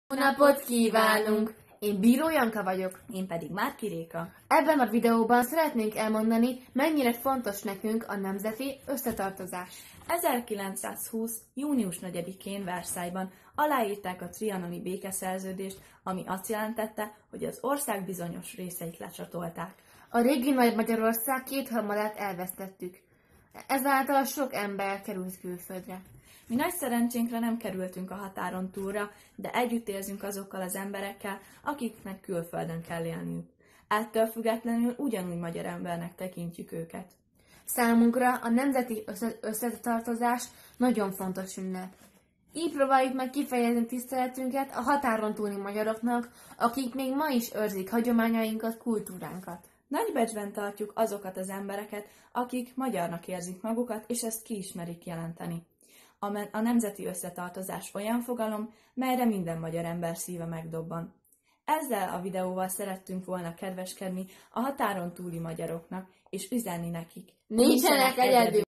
Bíró Janka, Márki Réka - Barátnők az összetartozásról
Nemzeti Összetartozás Napja – „Milyen jó, hogy itt vagyunk” videópályázat
Alkotó: Bíró Janka, Márki Réka
Terület: Anyaország
Korosztály: 10-14 évesek